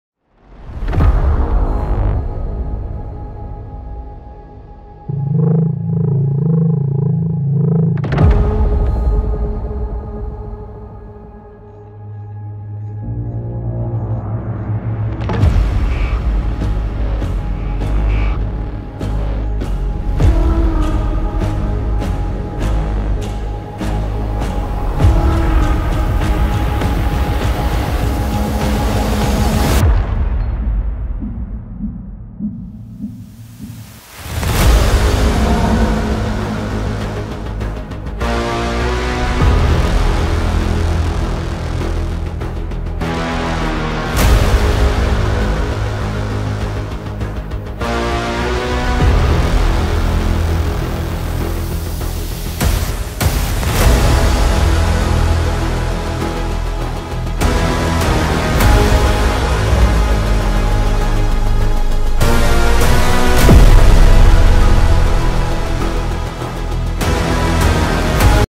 Attack from outside | AI cinematic video | runway gen-2 | midjourney
جميع ماتم هنا بخلاف الخلفيه الصوتيه هو Ai  يومين شغل

only!! the video is AI generated not the music
Gen-2 by Runway and midjourney